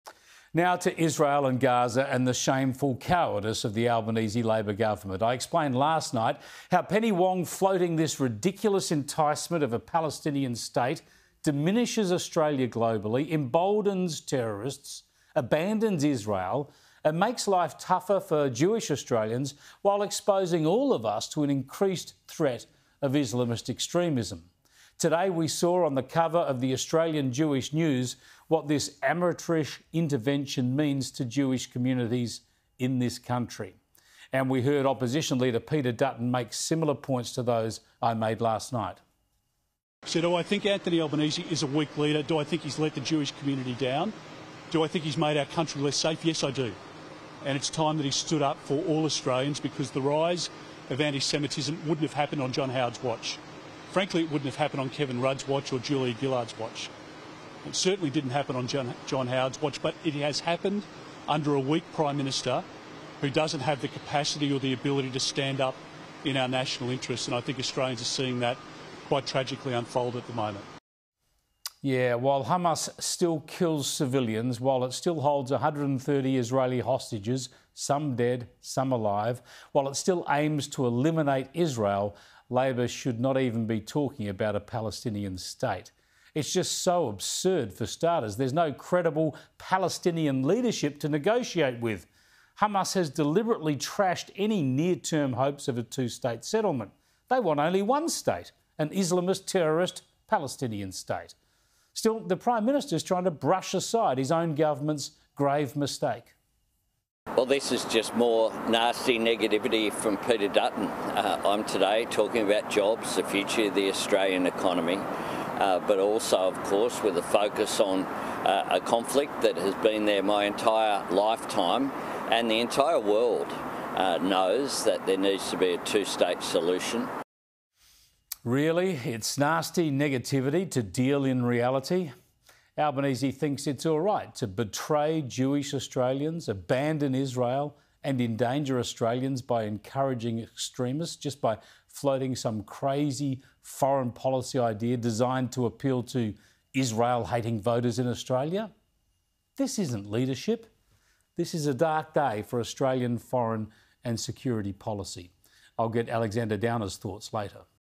Albanese government shows ‘shameful cowardice’ with Palestine call
Sky News host Chris Kenny has slammed the Albanese government’s “shameful cowardice” with Foreign Minister Penny Wong calling for a Palestinian state.

Addressing the ANU's National Security College on Tuesday evening, Senator Wong claimed a two-state solution was essential to sustain long-term peace in the region.

Ms Wong also asserted there was “no role for Hamas” in a future Palestinian state.

Speaking to Sky News Australia on Thursday, Prime Minister Anthony Albanese doubled down on the establishment of a two-state agreement being the path to peace for both Israelis and Palestinians.

“Albanese thinks it's alright to betray Jewish Australians, abandon Israel, and endanger Australians by encouraging extremists just by floating some crazy foreign policy idea designed to appeal to Israel-hating voters in Australia?” Mr Kenny said.

“This is not leadership. This is a dark day for Australian foreign and security policy.”